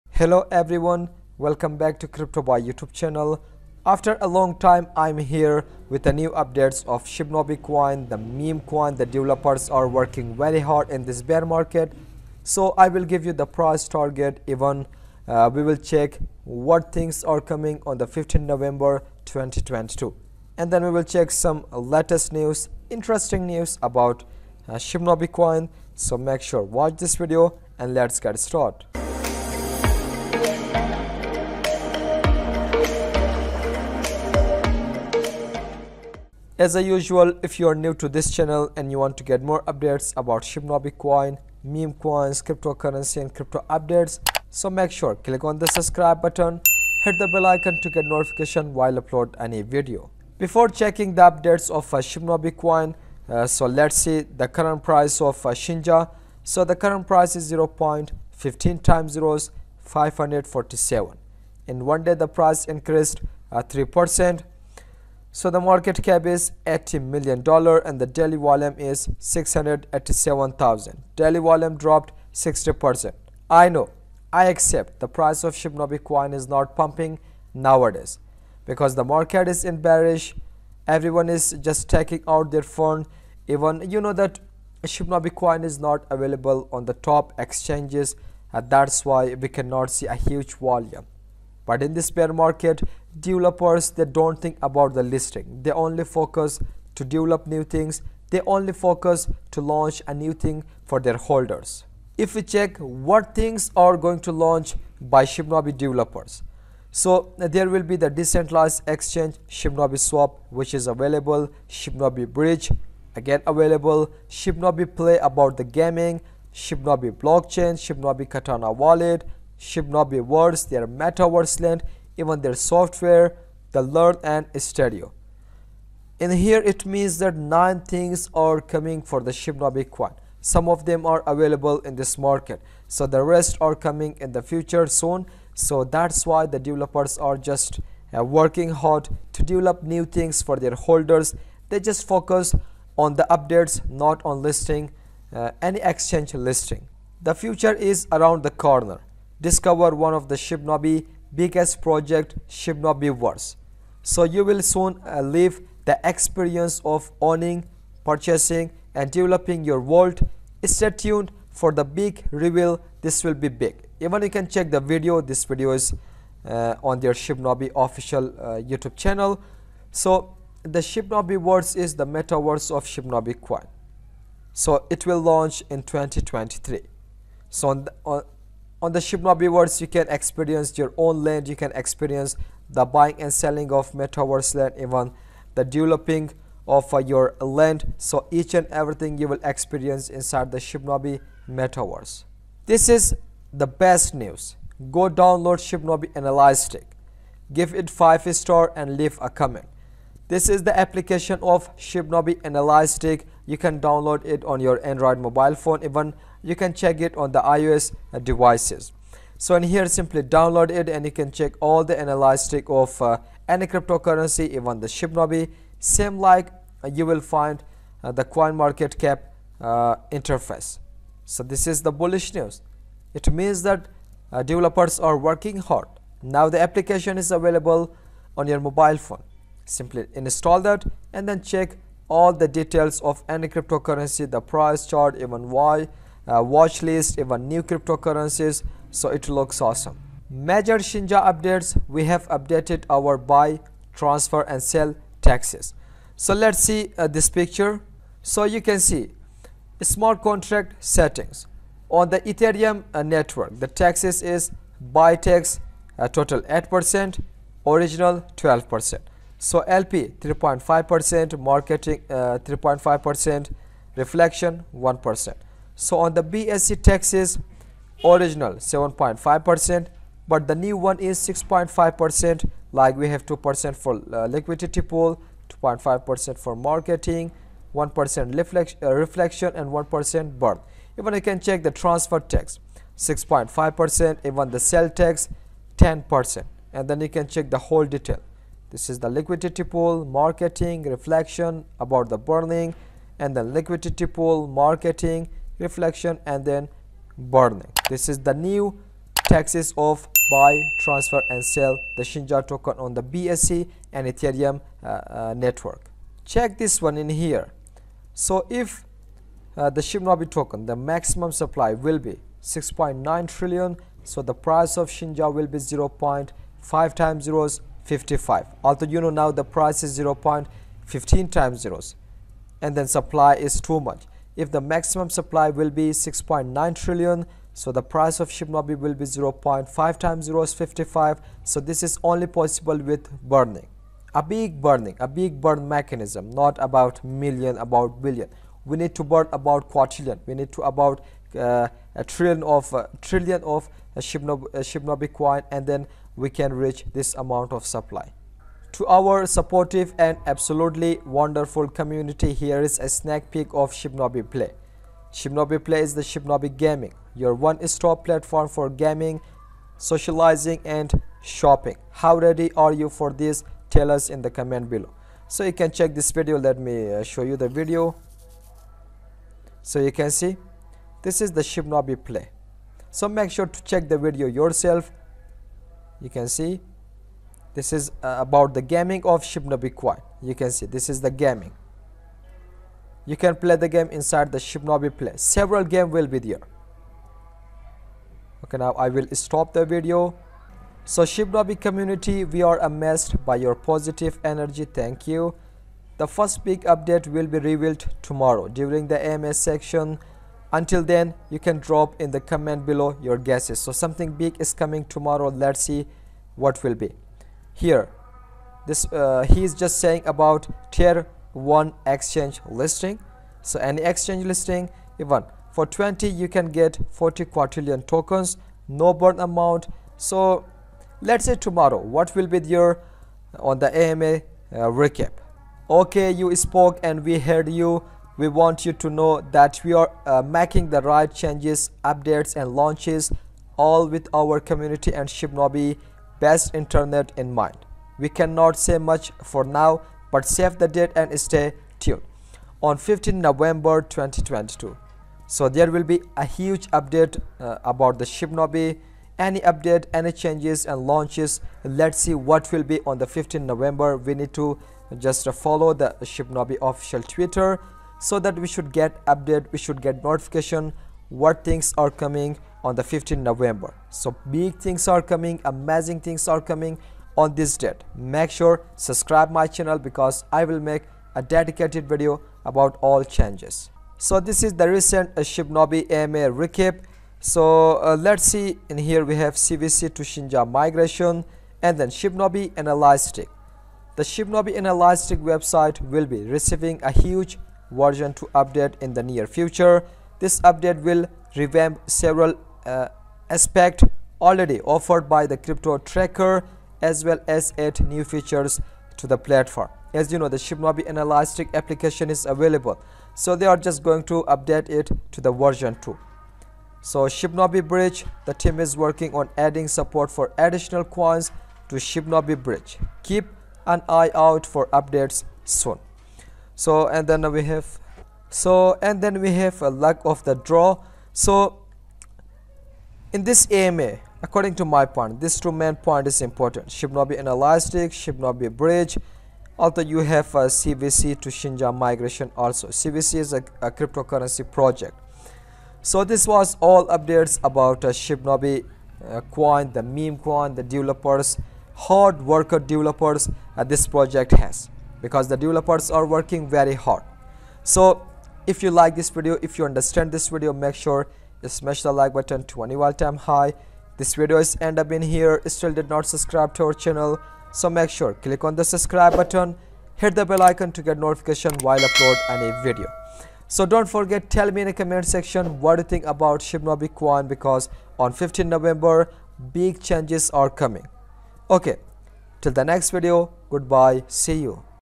Shibnobi 15 Nov 2022 Big Day | Shibnobi Price $0.0000055 | Shibnobi Analytics App
In this video, I will give you the latest news and updates on Shibnobi Memecoin. On 15 Nov 2022 the big day for Shinja. Besides that, I will show the Shibnobi Analytics application.

But, we are trying our best to bring accurate video and technical analysis for you so you can make profits. It's your money and it will be your loss and profit.